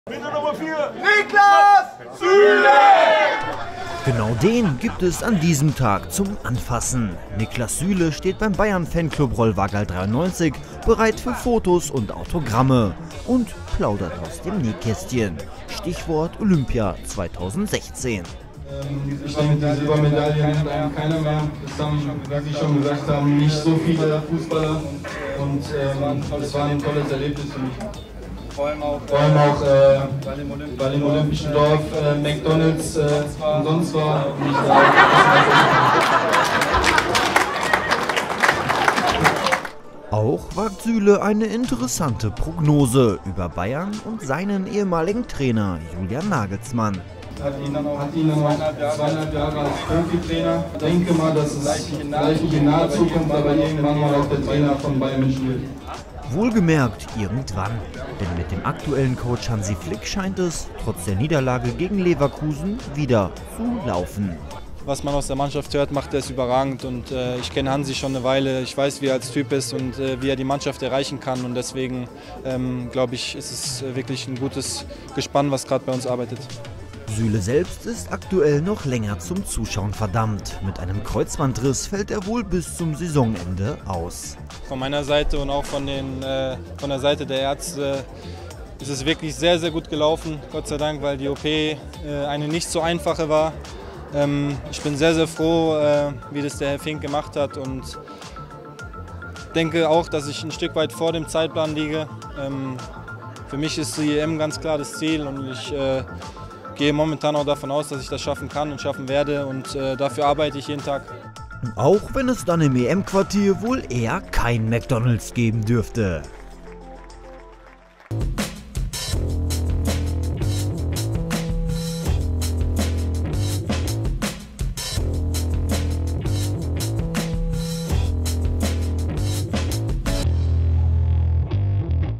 "Gratis McDonalds" - Süle sorgt für Lacher | SPORT1
Bei den Olympischen Spielen 2016 in Rio de Janeiro gewann Niklas Süle die Silbermedaille. Der Verteidiger genoss in Brasilien auch den kostenlosen McDonalds, wie er nun zugibt.